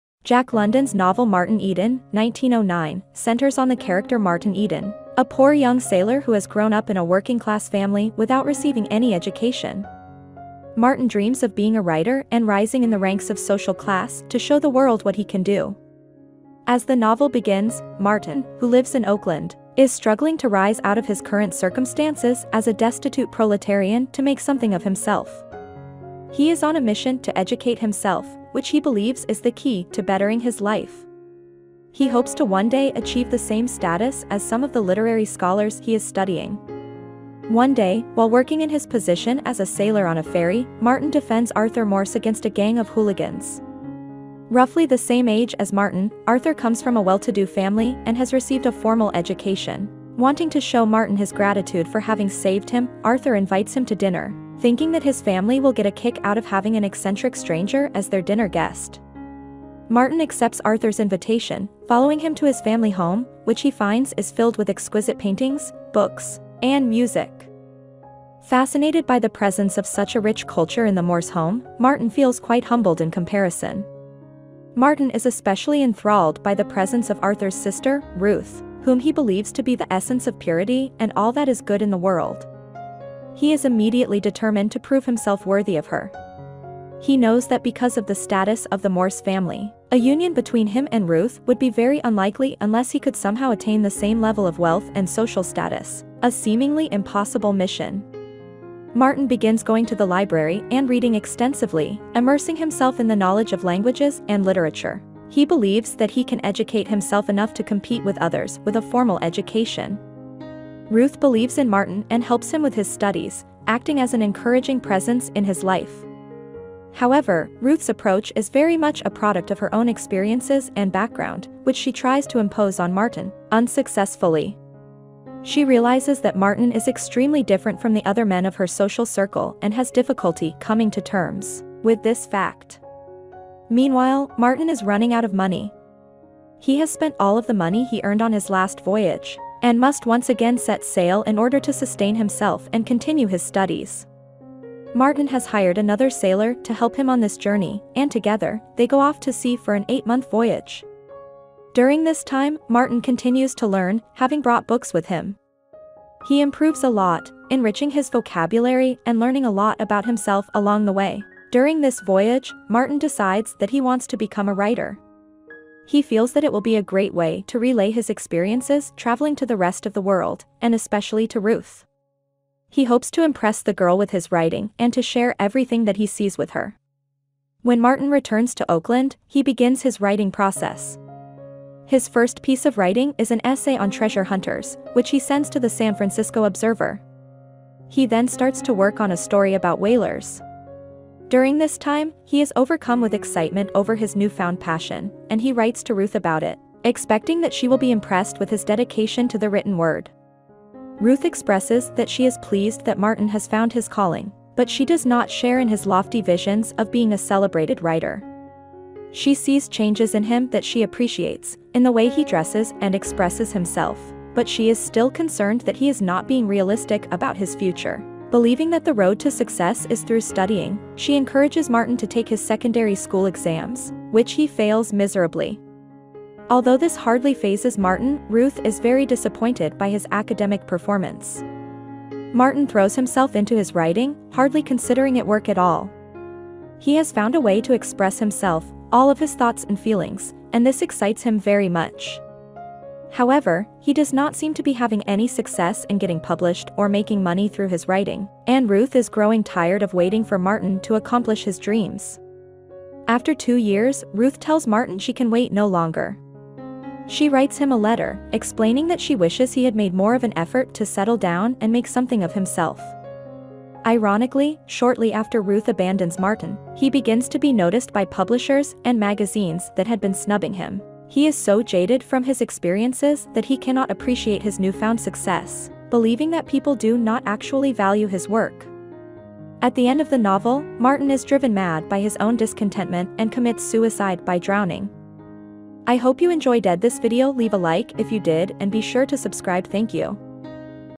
Plot summary, “Martin Eden” by Jack London in 5 Minutes - Book Review
Jack London’s novel Martin Eden (1909) centers on the character Martin Eden, a poor young sailor who has grown up in a working-class family without receiving any education. Martin dreams of being a writer and rising in the ranks of social class to show the world what he can do.